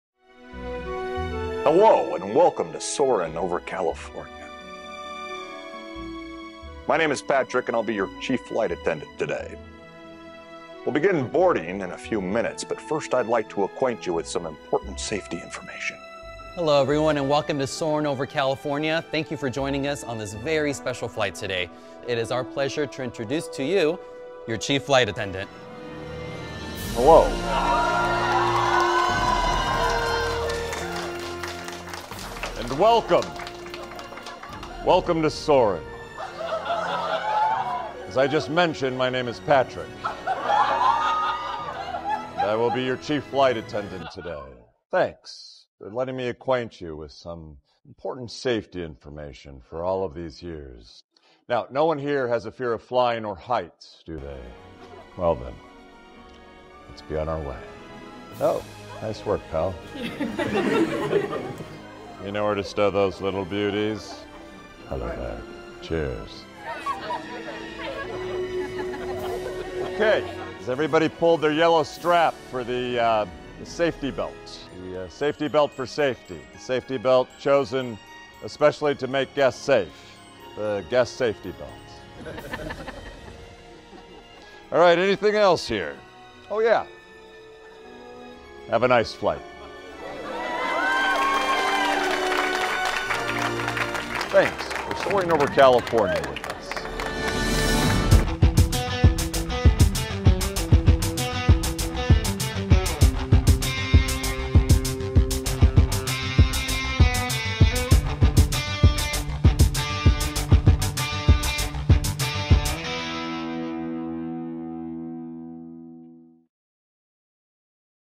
Patrick Warburton Returns as Chief Flight Attendant at Soarin’ Over California
Watch as guests get an unexpected guest before their ride on Soarin' Over California! It's like seeing an old friend! Thanks, pal.